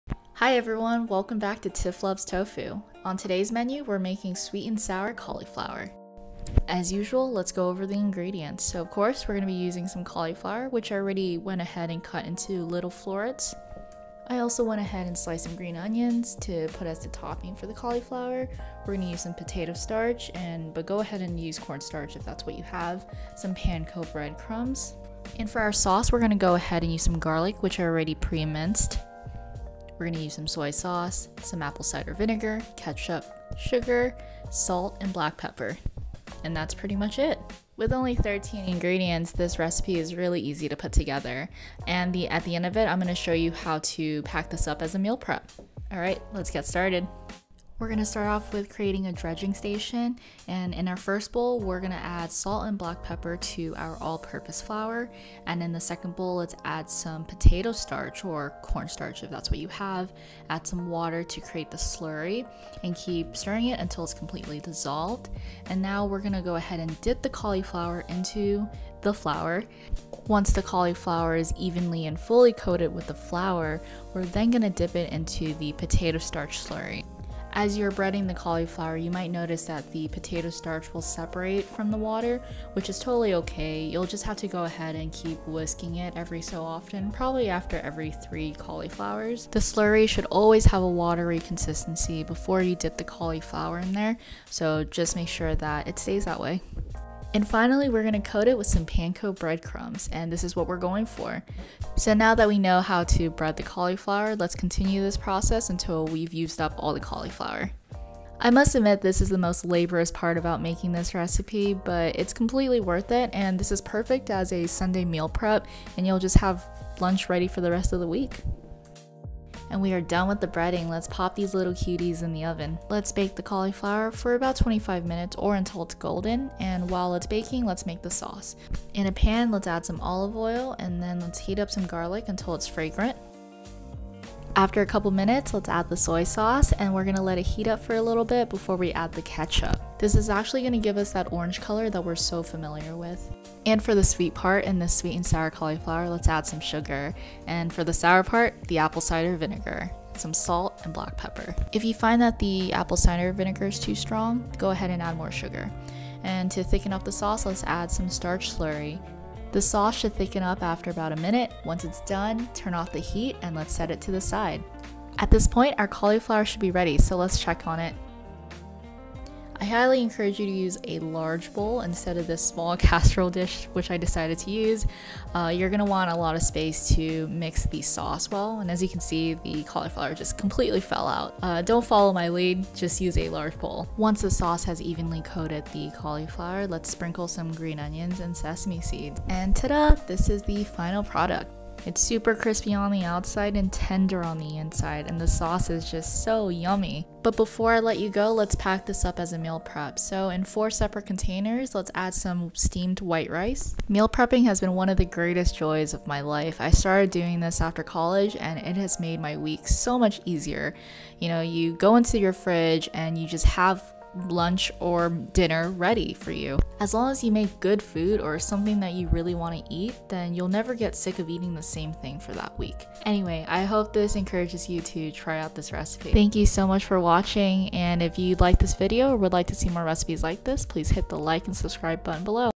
The most delicious cauliflower recipe
Cauliflower is the theme for this week! Here’s a cauliflower recipe that shows just how versatile this vegetable can be. Crispy on the outside, tender on the inside and smothered in an irresistible saucey sauce. Although this is perfect as a meal prep, as mentioned in the video, you might end up wanting to eat it all after you take a bite. Enjoy!

Ingredients

Cauliflower
1 cauliflower, cut into florets
1 cup all-purpose flour
¼ cup potato or corn starch (save 1 tbsp to use for the sauce)
2 tbsp water (add 1 tsp at a time until reaching loose consistency)
2-3 cups panko bread crumbs
1 tbsp salt
1 tbsp ground black pepper

Sauce
1 tbsp vegetable oil
3 cloves garlic, minced
2/3 cup apple cider vinegar
1/4 cup soy sauce
1/2 cup sugar
1/2 cup ketchup
1 tbsp potato or cornstarch + 1 tsp water (to create slurry)

Toppings
1 green onion, roughly chopped
1 tbsp sesame seeds

Direction

1. Preheat the oven to 400°. Prep the cauliflower, garlic and green onions and set to the side.
2. Set up a dredging station. In one bowl, add the flour, salt and pepper. In another bowl, create the starch slurry. The potato or cornstarch slurry will separate so plan to whisk it every so often. Finally, in the last bowl, add the panko breadcrumbs. Dip cauliflower in the flour, then the starch slurry, and finally, the panko breadcrumbs until fully coated. Transfer the cauliflower to a parchment paper lined baking sheet. Repeat this process until all the cauliflower has been breaded.
3. Bake the cauliflower until slightly golden and crispy, about 20 to 25 minutes.
4. Meanwhile cauliflower is baking, make the sauce. In a pan, heat up the vegetable oil over medium heat. Add the garlic and let it cook for about 2 minutes. Next, add the apple cider vinegar, sugar, soy sauce and ketchup and stir to combine. Mix the water and starch together to create the slurry and then stir into the sauce. Once the sauce has thickened, turn off the heat and set aside.
5. Once the cauliflower has been baked, toss it in the sauce. Garnish with green onions and sesame seeds. Serve it with steamed rice, avocado slices, roasted peanuts, and fresh bell pepper for a complete meal!